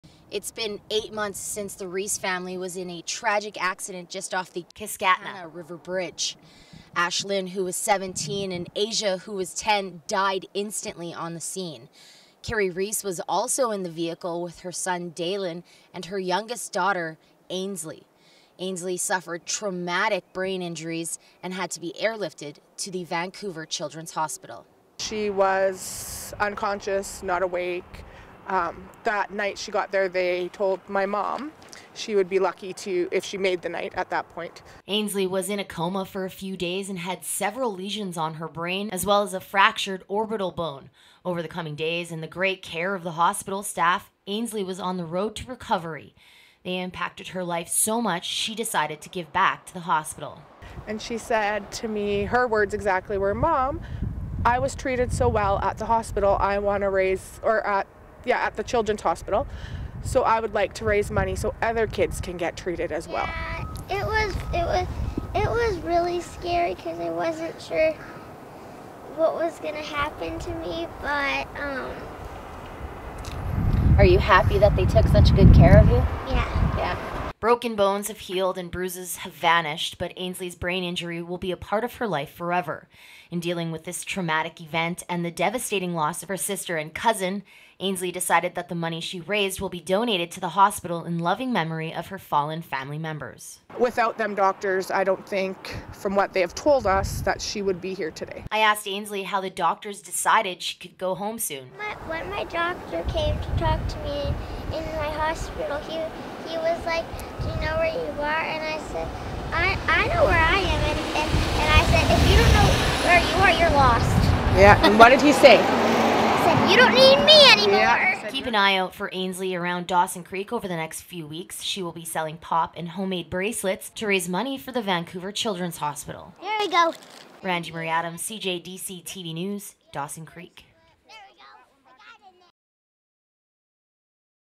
Dawson Creek girl turns tragedy into a touching tribute
One local Dawson Creek girl is raising money for the Vancouver Children Hospital.

CJDC-TV's Randi-Marie Adams speaks with Ainsley Riise, who spent time there after being involved in a tragic accident on the Kiskatinaw River bridge.